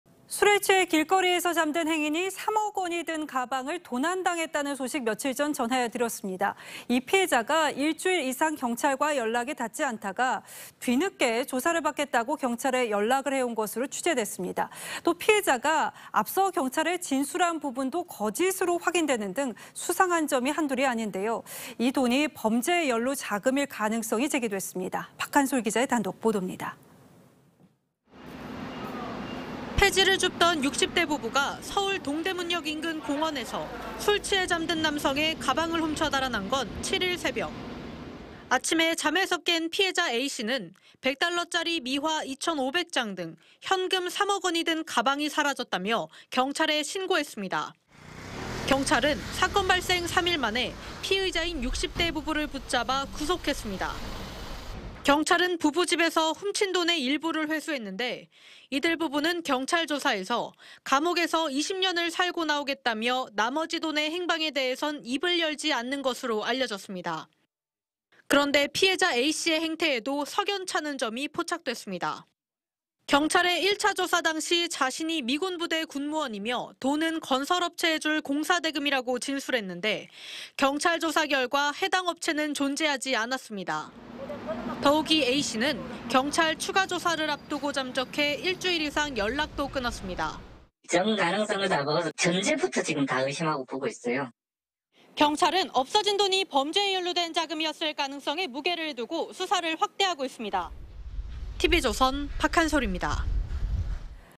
[단독] 3억 피해자, 한때 연락끊겨…'범죄연루' 수사 확대
술에 취해 길거리에서 잠든 행인이 3억 원이 든 가방을 도난당했다는 소식, 며칠 전 전해드렸습니다. 이 피해자가 일주일 이상 경찰과 연락이 닿지 않다가 뒤늦게 "조사를 받겠다"고 경찰에 연락을 해온 것으로 취재됐습니다. 또 피해자가 앞서 경찰에 진술한 부분도 거짓으로 확인되는 등 수상한 점이 한둘이 아닌데요, 이 돈이 범죄 연루 자금일 가능성이 제기됐습니다.